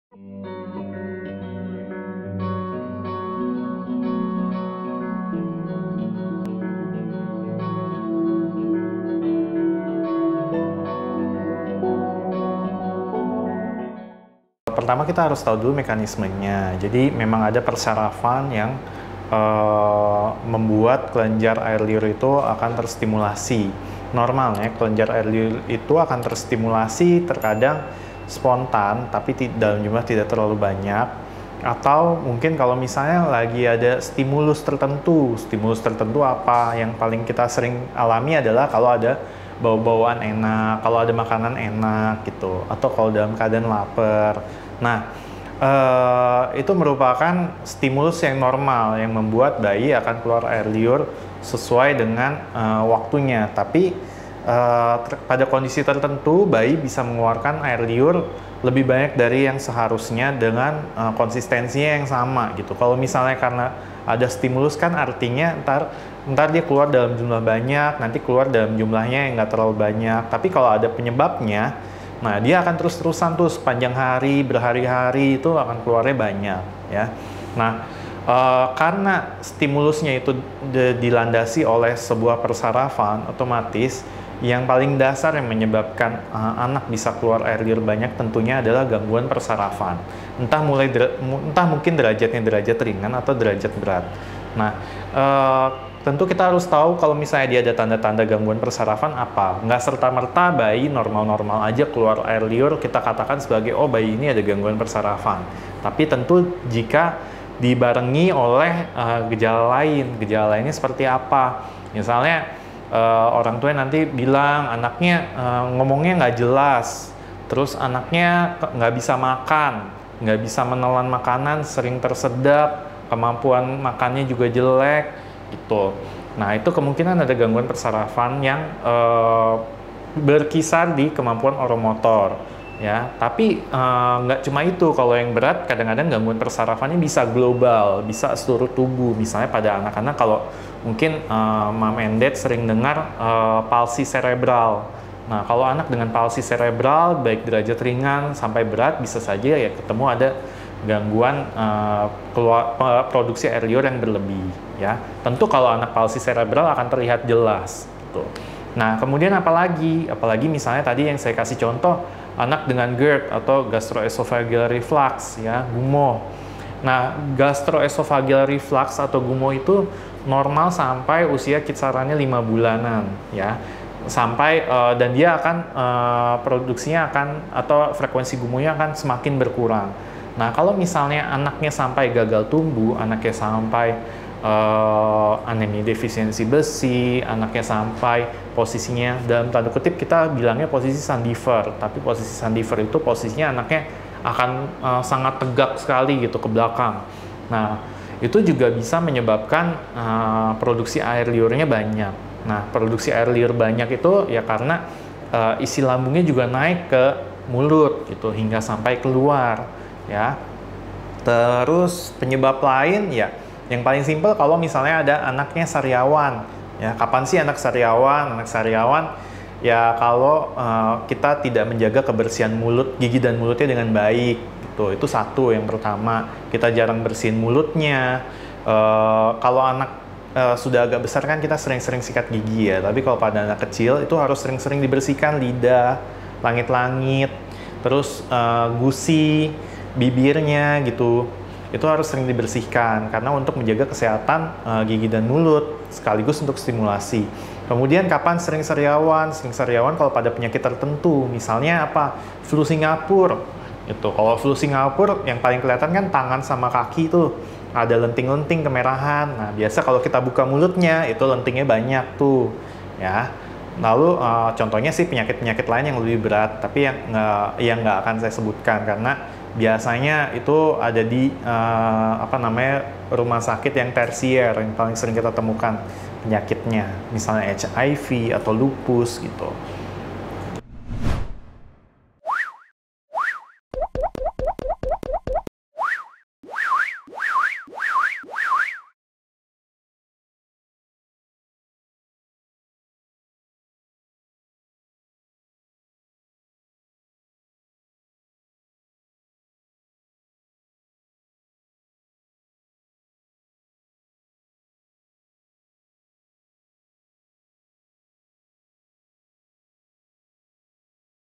Penjelasan Lengkap dari Dokter Anak Mengenai Penyebab Ngiler pada Bayi
Meski terlihat sederhana, ternyata bayi yang mengiler disebabkan oleh berbagai penyebab atau indikasi tertentu Moms. Dokter Spesialis Anak dari RSU Bunda, dr. Anthony Christian Darmawan menjelaskan secara lengkap untuk Moms.

00:00 Intro
00:14 Apa penyebab ngiler pada bayi?

ADVERTISING